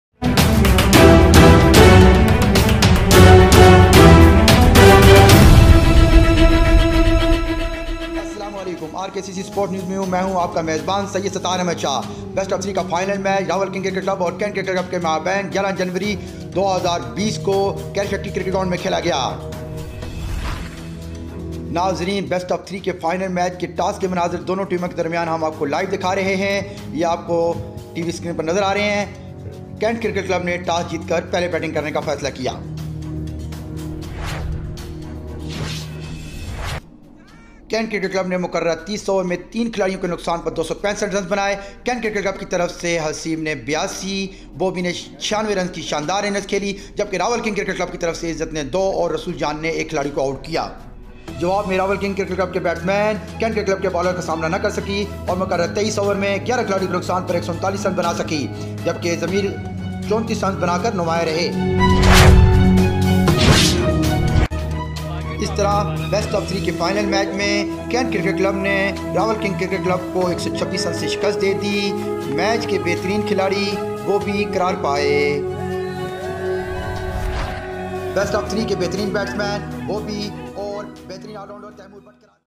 Best of Three Final Kent Cricket Club Vs Rawal Kings Cricket 11-01-2020 at Carriage Factory Ground
Rawal Kings Club vs Kent Club Rwp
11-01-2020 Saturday
30 Over’s Match
Rawal Kings Ground Carriage Rwp
Kent Club won the toss and Batting First.

Kent Club Batting.
265/3 in 30 Overs.

Bobi
96* Runs 52 Balls
Haseeb
82* Runs 69 Balls
Taimor But
14 Runs 11 Balls
Nouman
17 Runs 24 Balls

Rawal Kings Club Bowling

Izar
4-0-25-2 Wkts
Sharif
6-0-50-0 Wkts
Rasool Jan
6-0-63-1 Wkts
Shafqat
6-2-55-0 Wkts
Hamza
6-0-46-0 Wkt

Rawal Kings Club
139/11 in 23 Overs

Zameer
34 Runs 32 Balls
Ameer Hamza
22 Runs 15 Balls
Ashfaq Hussain
16 Runs 13 Balls
Talah
14 Runs 14 Balls

Kent Club Bowling

Ifti
4-0-24-2 Wkts.
Babar
3-0-8-3 Wkts
Faisal
4-1-20-2 Wkts
Taimor
3-0-21-1 Wkts

Kent Club won by 126 Runs. Man of the match Bobi and Taimor But.